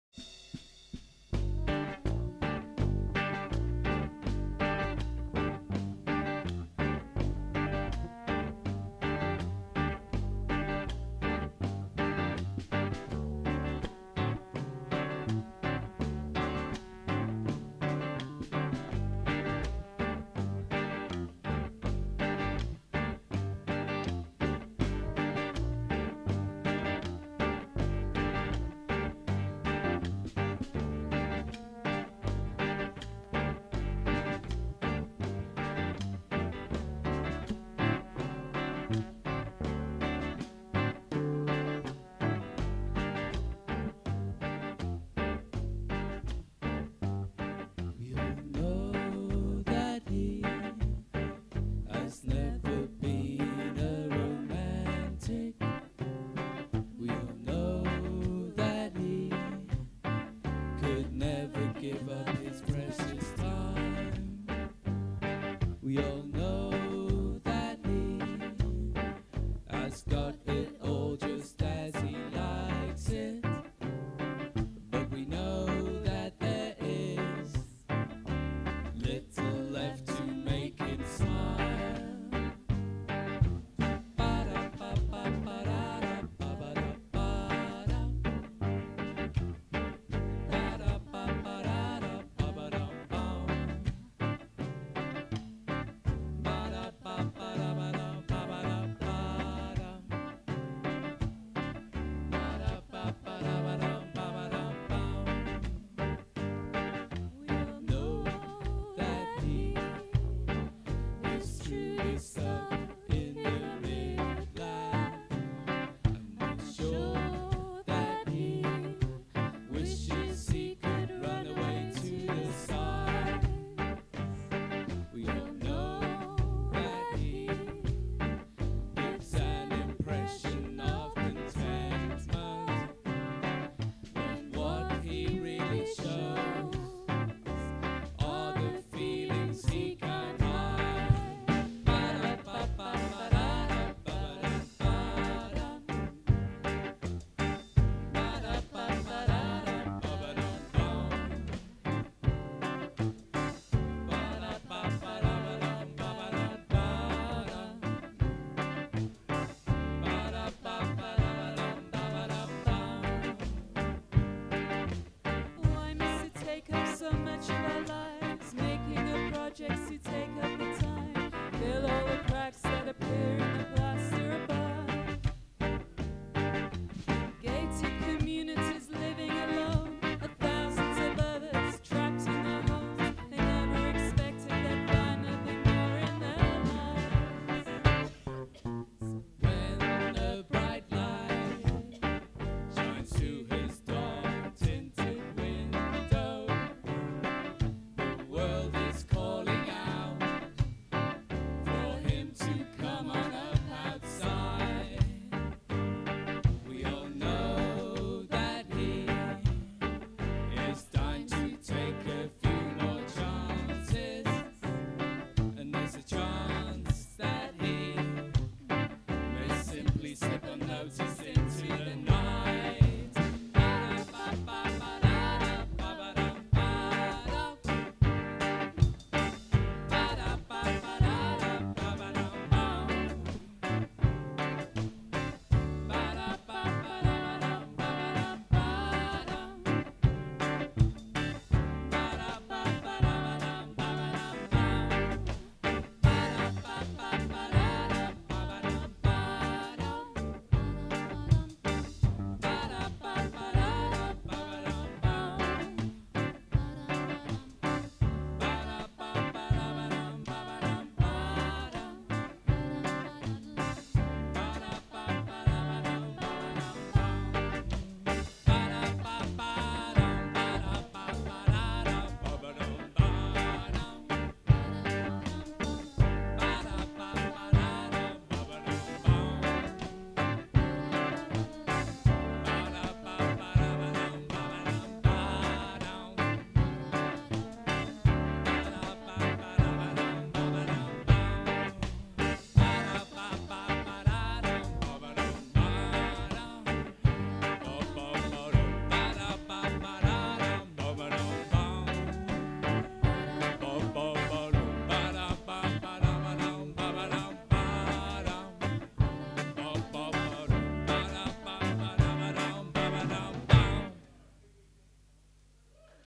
Troubadour Hook Live at The Globe
Troubadour Hook Live at The Globe at Hay (Coexist Music Collective), October 29th 2010